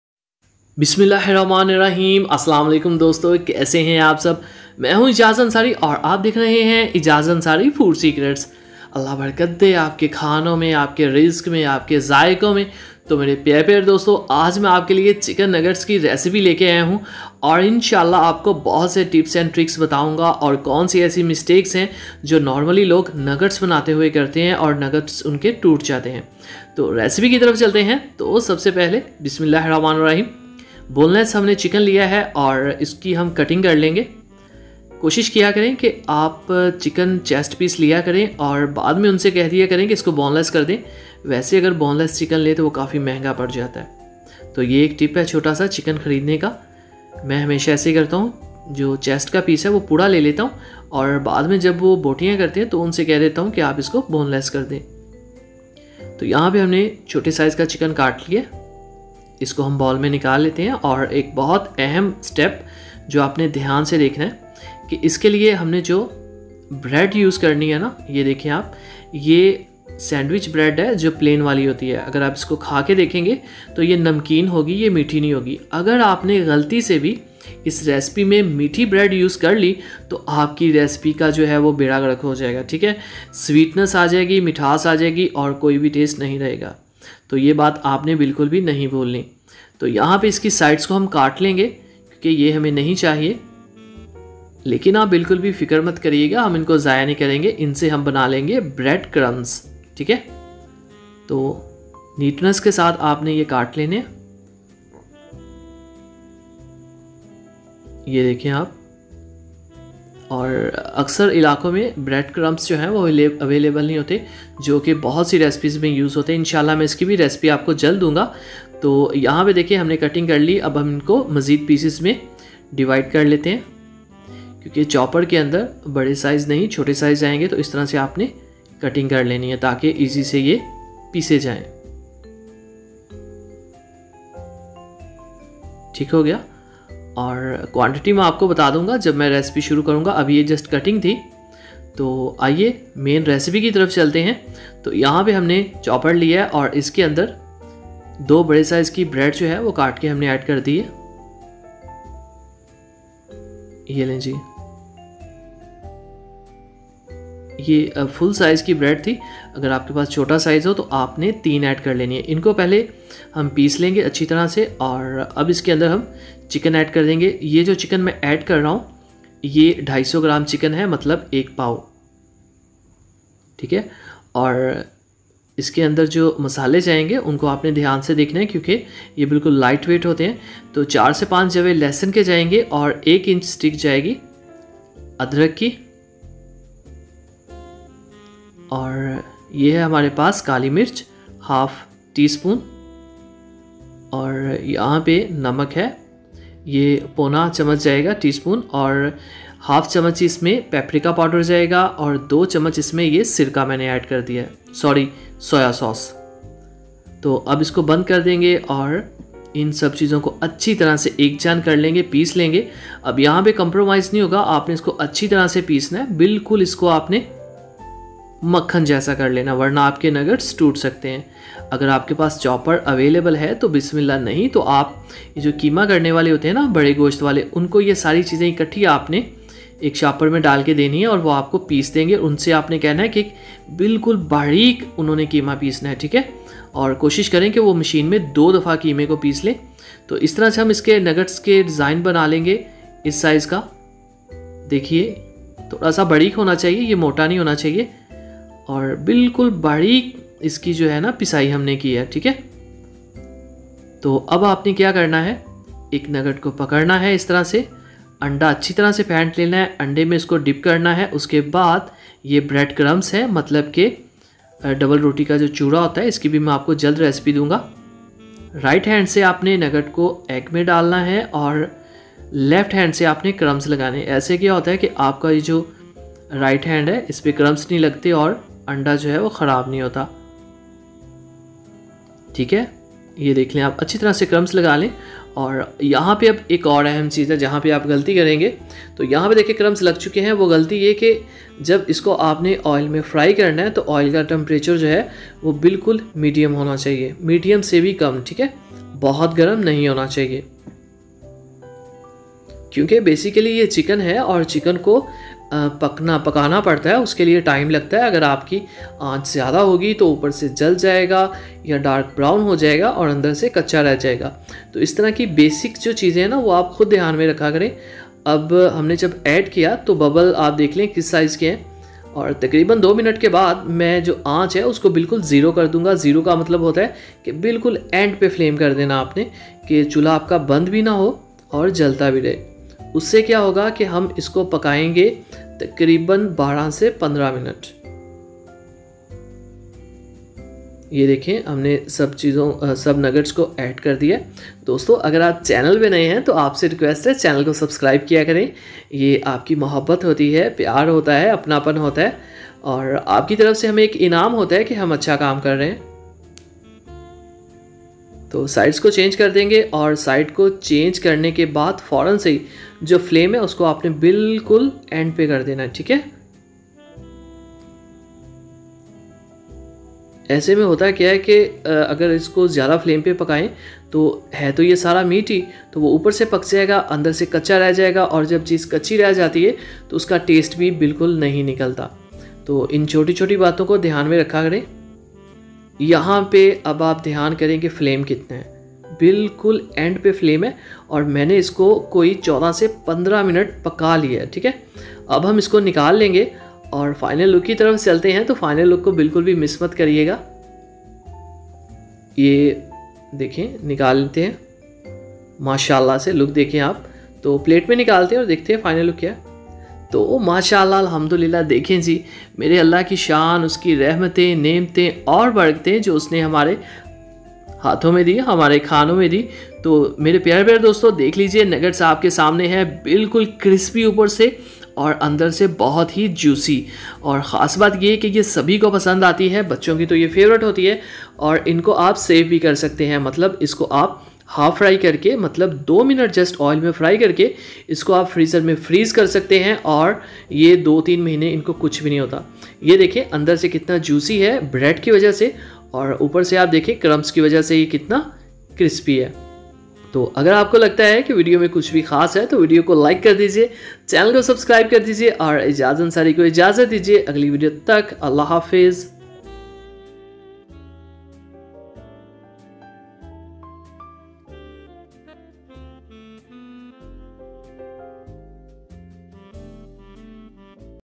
Chicken Nuggets Recipe By Ijaz Ansari || گھر پر چکن نگٹس بنانے کا طریقہ || Easy Homemade Nuggets
aaj me ap k lye chicken Nuggets homemade recipe laya hun. Nuggets Recipe in urdu. chicken nuggets easy recipe. chicken nuggets best recipe.